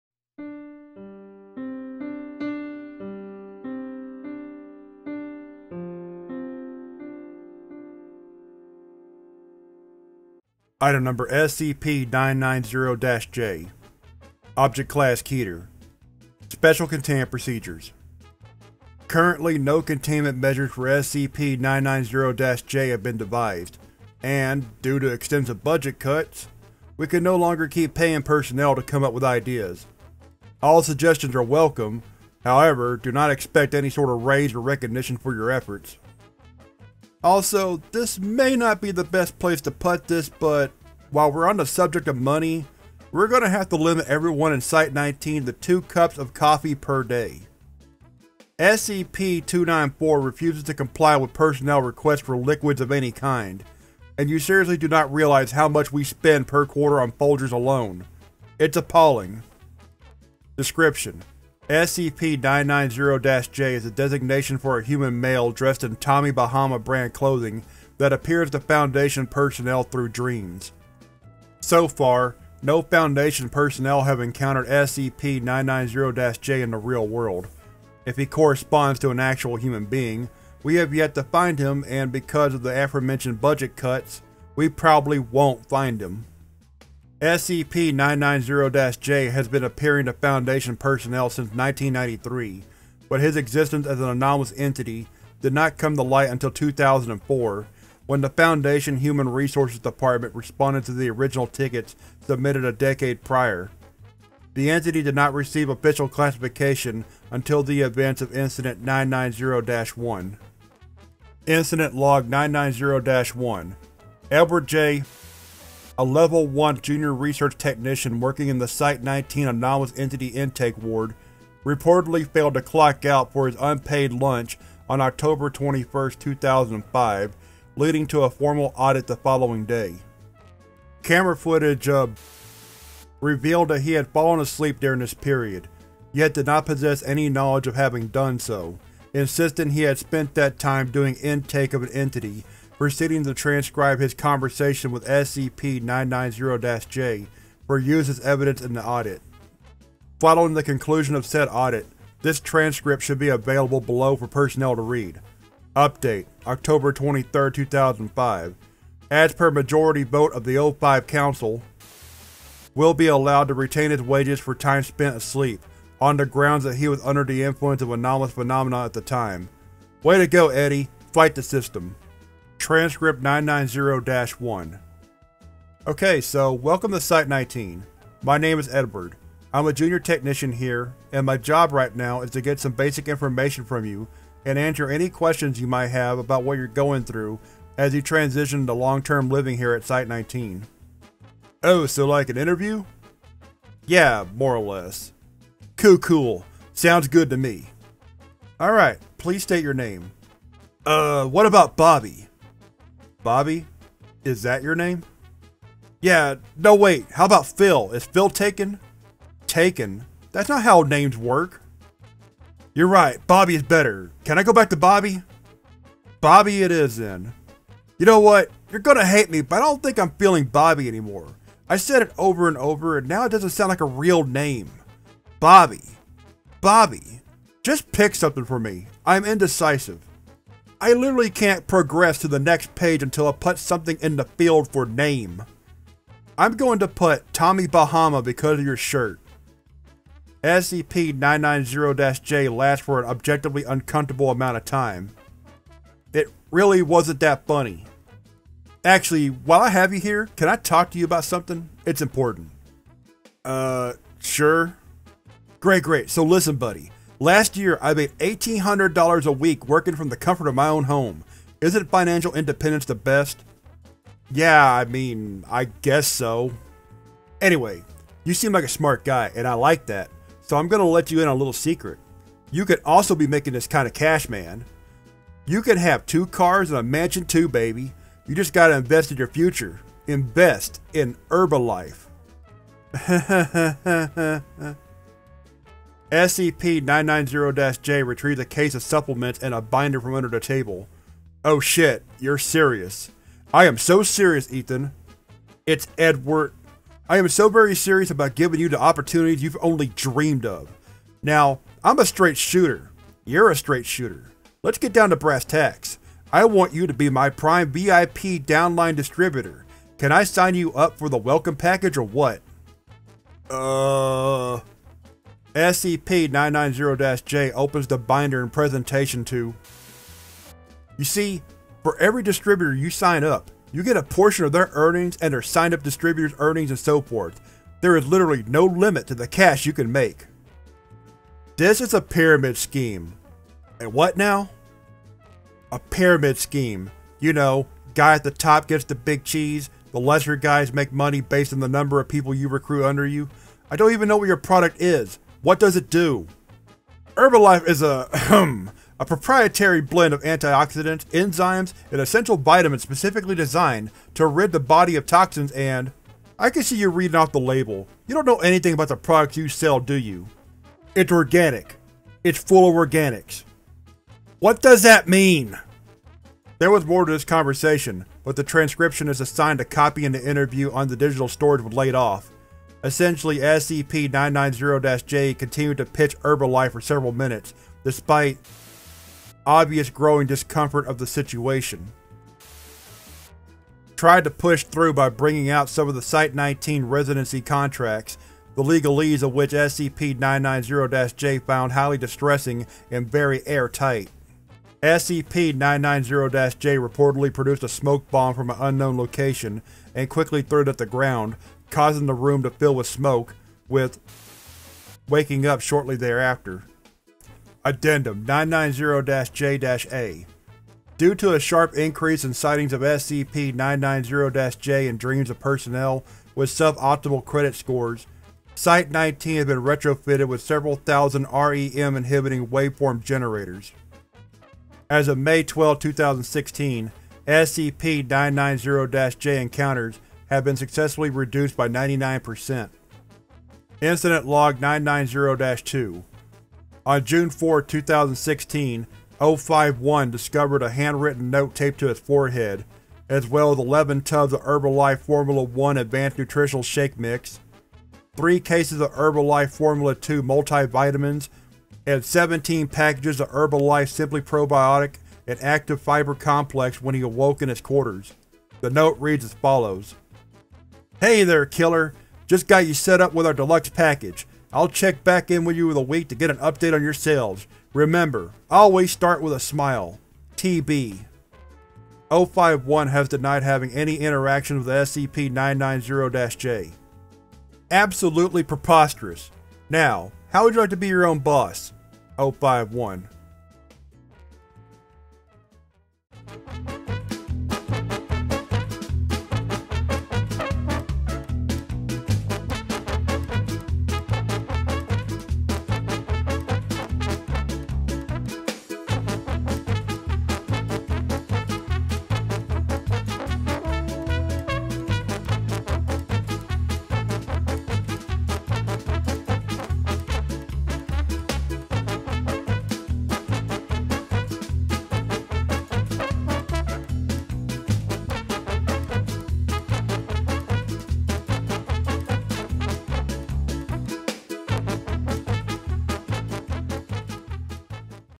SCP-990-J Dream Dude | joke scp
This SCP Foundation wiki joke reading is about SCP 990 J Dream Dude is a joke scp parody of SCP-990 Dream Man. This humanoid entity wears Tommy-Bahama brand clothing and loves to sell pyramid schemes to unsuspecting dreamers.
SCP-990-J is the designation for a human male dressed in Tommy Bahama-brand clothing that appears to Foundation personnel through dreams. So far, no Foundation personnel have encountered SCP-990-J in the real world; if he corresponds to an actual human being, we have yet to find him, and because of the aforementioned budget cuts, we probably won't find him.

SCP-990-J has been appearing to Foundation personnel since 1993 but his existence as an anomalous entity did not come to light until 2004, when the Foundation Human Resources Department responded to the original tickets submitted a decade prior. The entity did not receive official classification until the events of Incident 990-1.

SCP reading video created by Eastside Steve of The Eastside Show!
Written by Billith